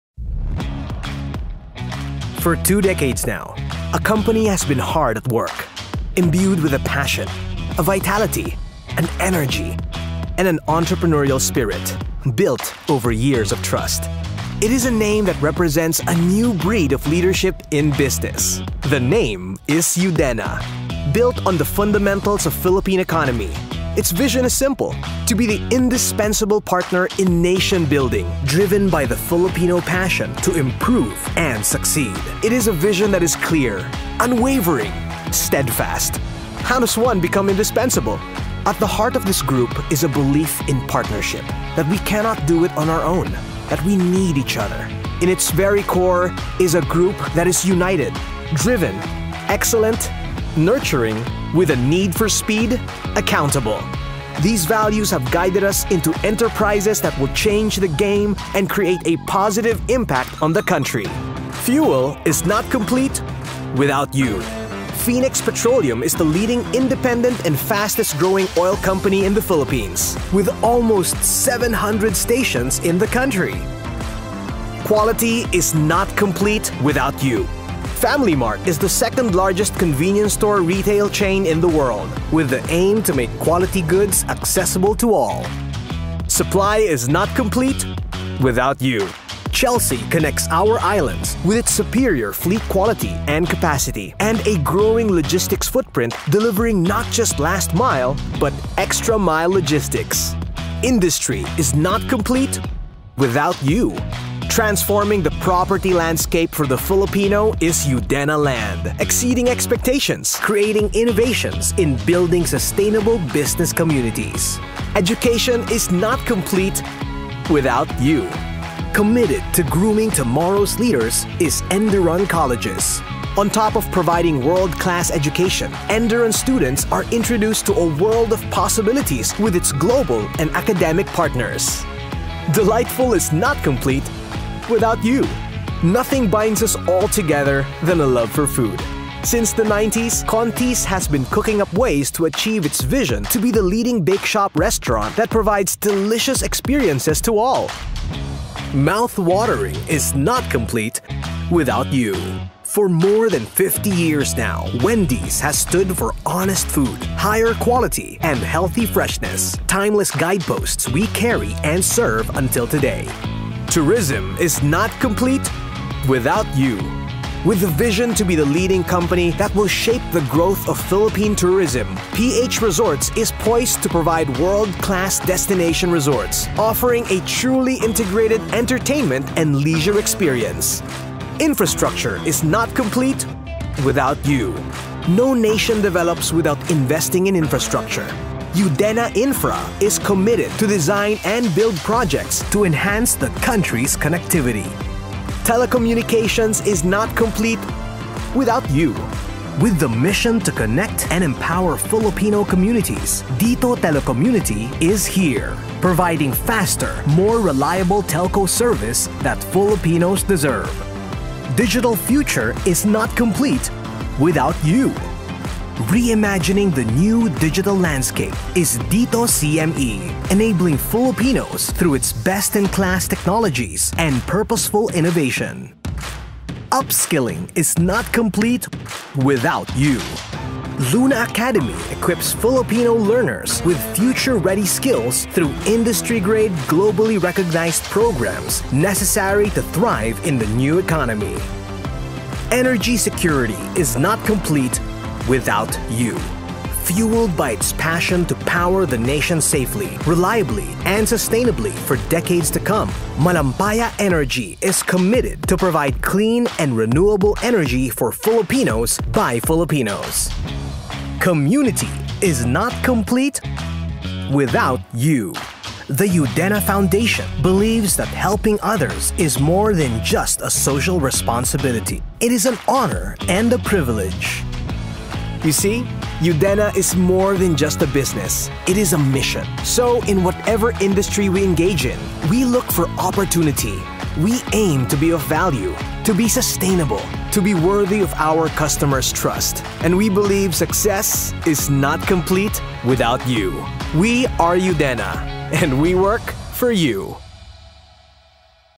UDENNA Corporation
For 20 years now, a company has been hard at work. Driven by its vision to be an indispensable partner in nation building, driven by the Filipino passion to improve and succeed — UDENNA is more than just a business. It is a mission.
We are UDENNA and we work for U!